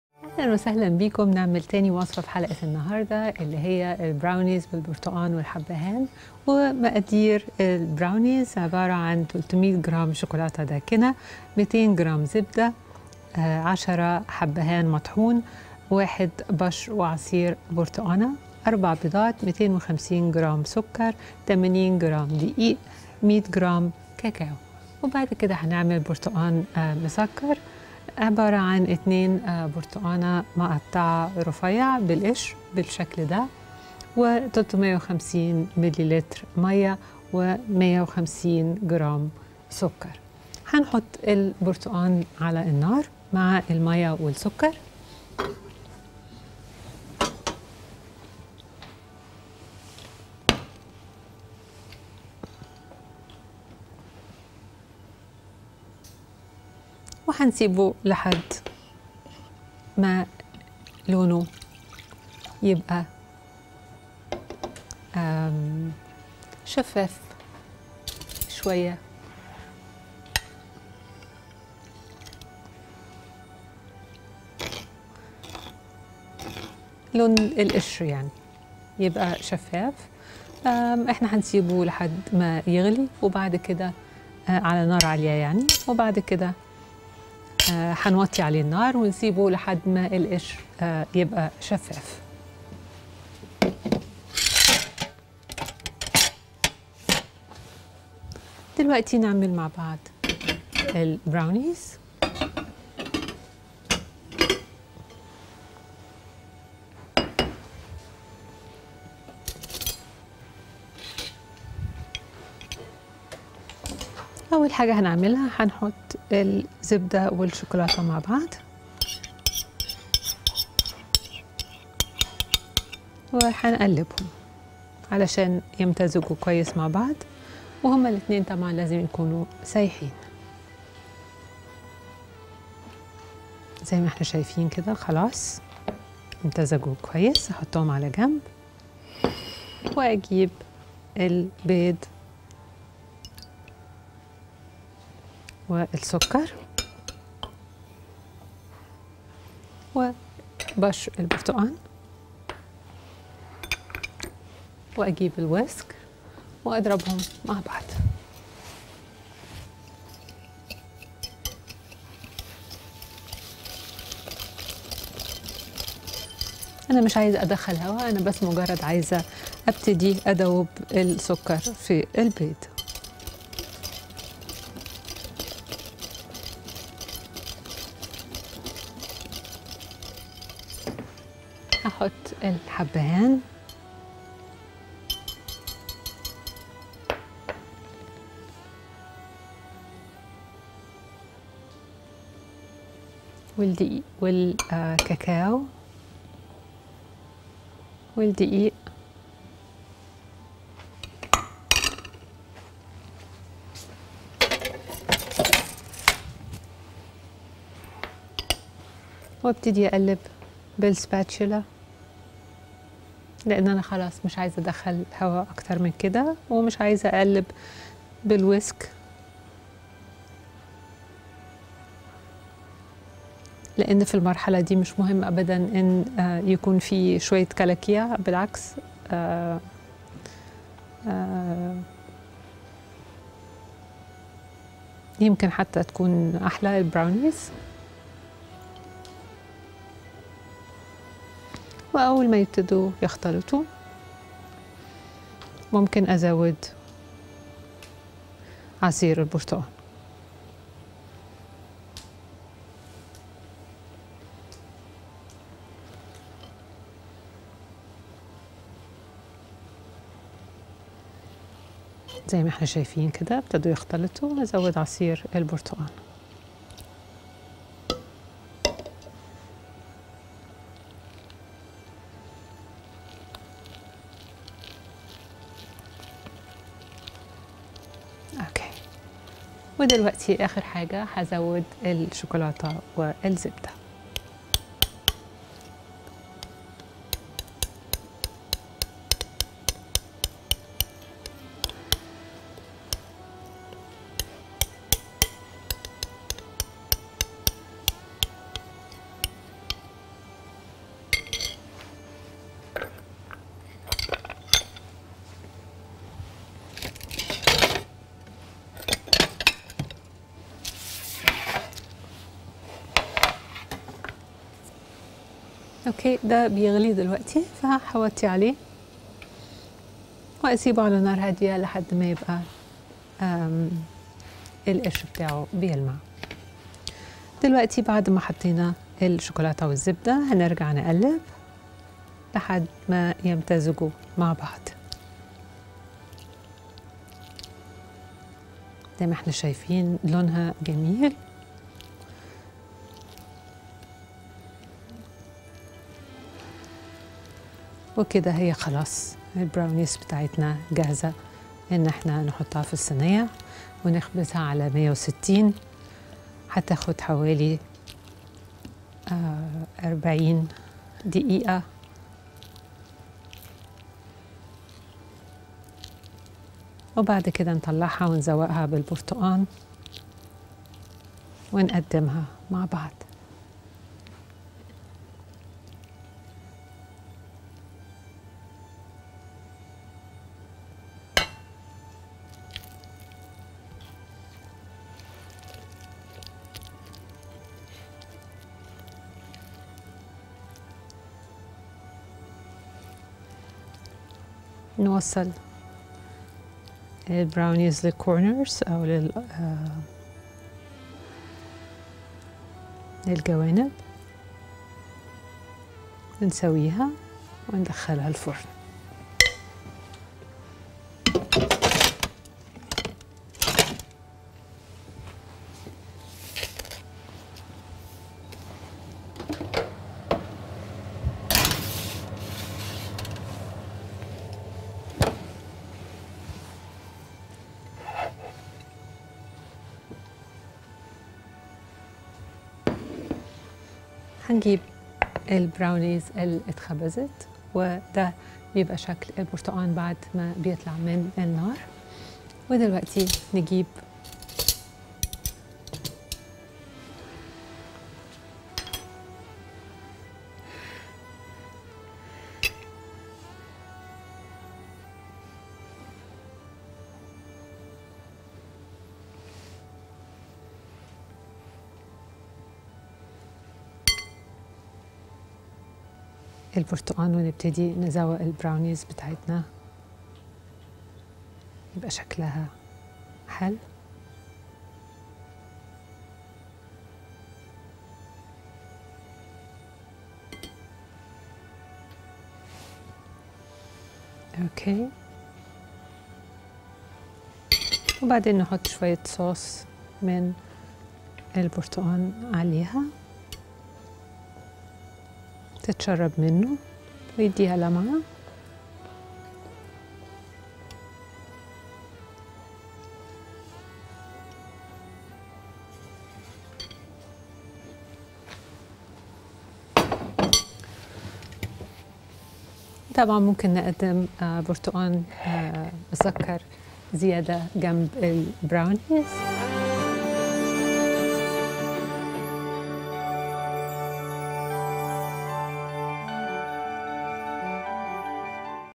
براونيز البرتقال المسكر والحبهان | نرمين هنو
طريقة عمل براونيز البرتقال المسكر والحبهان مع نرمين هنو في زي السكر